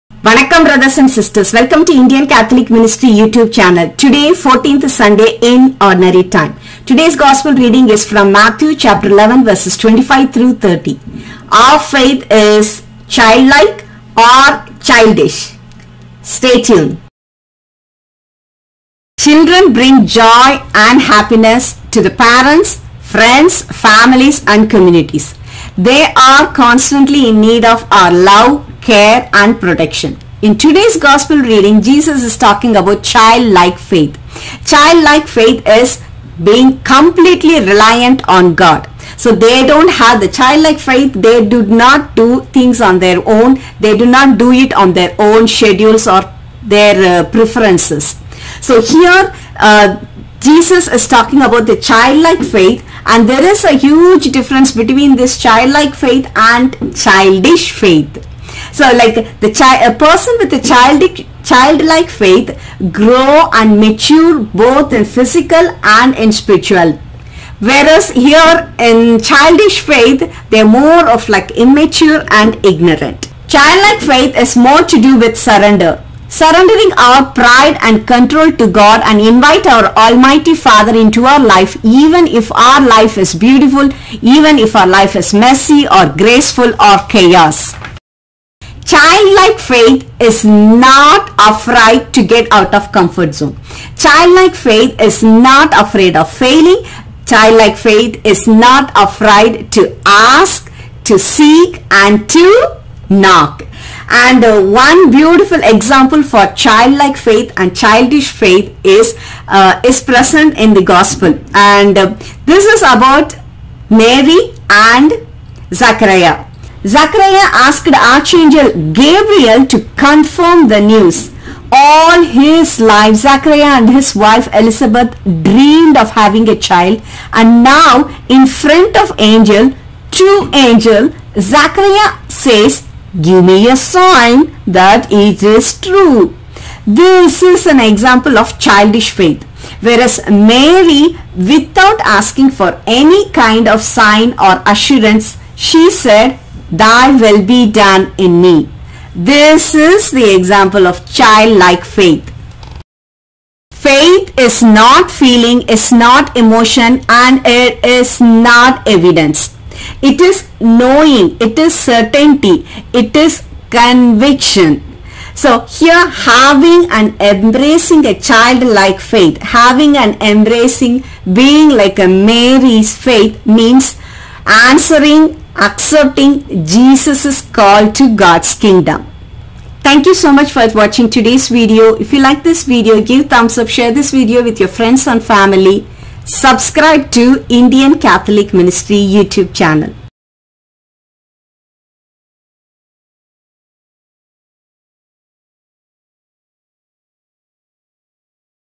Childlike Faith. 14th Sunday in Ordinary Time. Mt 11: 25-30. Sunday Gospel. Mary and Zacharias.
Childlike Faith. 14th Sunday in Ordinary Time. Mt 11: 25-30. Sunday Gospel. Childlike faith Vs Childish Faith. The faith of Mary and Zacharias.

At that time Jesus exclaimed:
“I give praise to you, Father, Lord of heaven and earth,
for although you have hidden these things
from the wise and the learned
you have revealed them to little ones.
Yes, Father, such has been your gracious will.
All things have been handed over to me by my Father.
No one knows the Son except the Father,
and no one knows the Father except the Son
and anyone to whom the Son wishes to reveal him.”
“Come to me, all you who labor and are burdened,
and I will give you rest.
Take my yoke upon you and learn from me,
for I am meek and humble of heart;
and you will find rest for yourselves.
For my yoke is easy, and my burden light.”

Credits for images:
jeffjacobs1990 - Pixabay